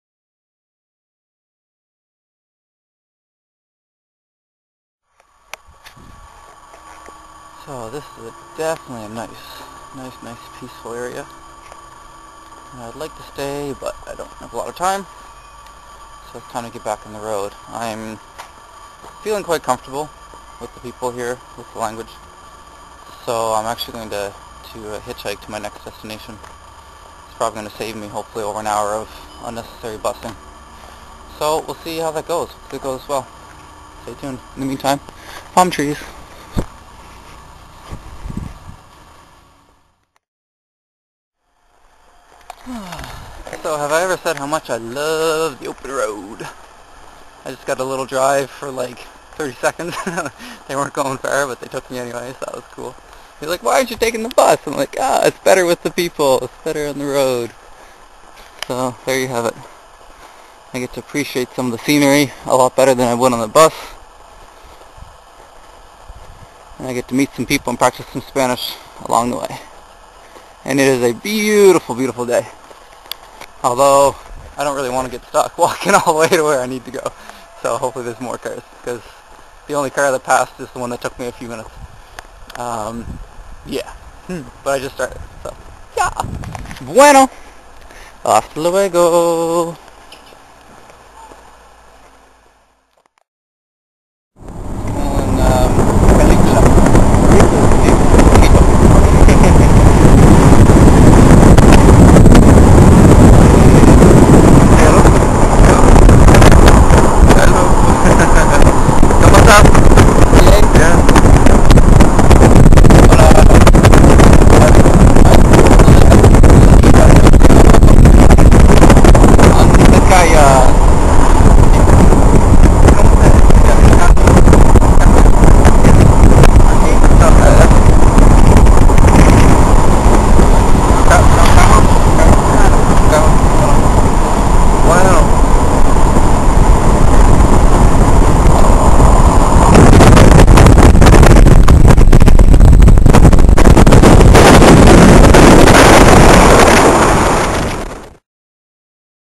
Hitchhiking in El Salvador
Good ol' open roads waiting for me. Not many other places I'd rather be. January 2010.